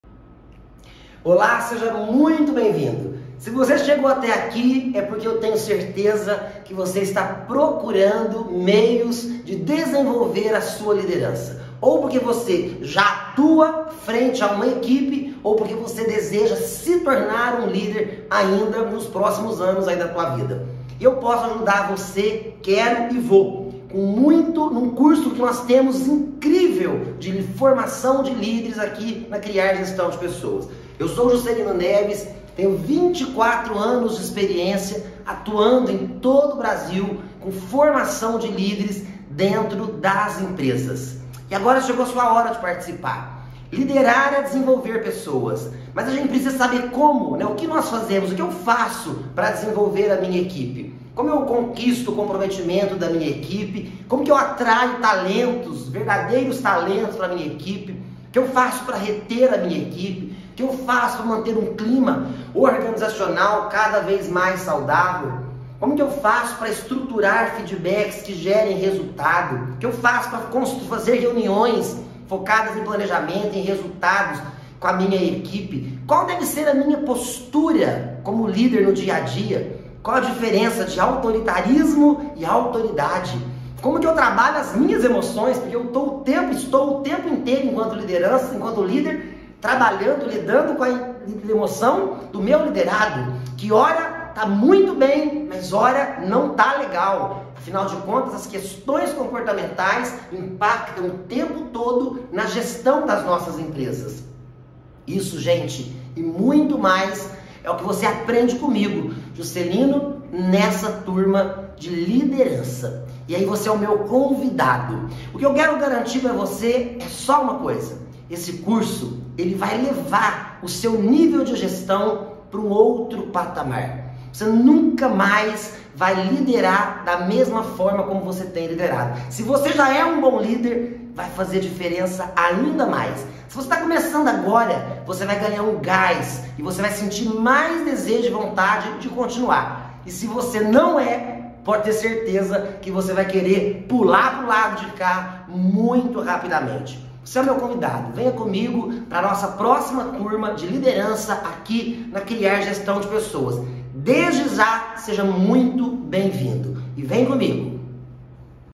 Desenvolvimento de Líderes 2023
Um curso completo para você que quer desenvolver ou melhorar suas habilidades de líder.